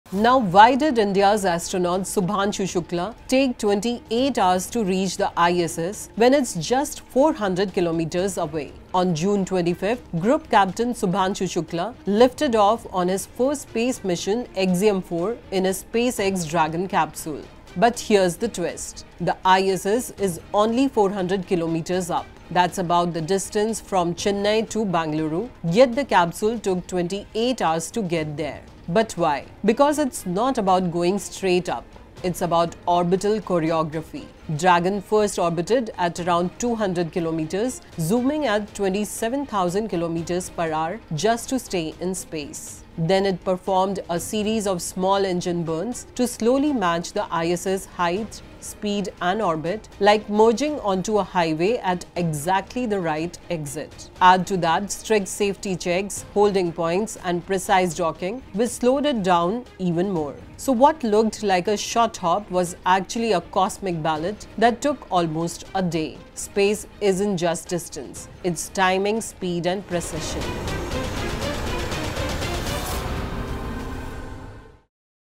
ISS is Just Chennai–Bengaluru Away! So Why it take 28 Hrs? | NEXT IAS
Why did India’s astronaut Shubhanshu Shukla take 28 hours to reach the ISS — when it’s just 400 km away?
On June 25, Group Captain Shubhanshu Shukla lifted off on his first space mission — Axiom-4 — in a SpaceX Dragon capsule. But here’s the twist: the ISS is only 400 km up — that’s about the distance from Chennai to Bengaluru — yet the capsule took 28 hours to get there.
Why? Because it’s not about going straight up — it’s about orbital choreography. Dragon first orbited at around 200 km, zooming at 27,000 km/hr — just to stay in space. Then, it performed a series of small engine burns to slowly match the ISS’s height, speed, and orbit — like merging onto a highway at exactly the right exit. Add to that strict safety checks, holding points, and precise docking — which slowed it down even more. So, what looked like a short hop was actually a cosmic ballet that took almost a day. Space isn’t just distance — it’s timing, speed, and precision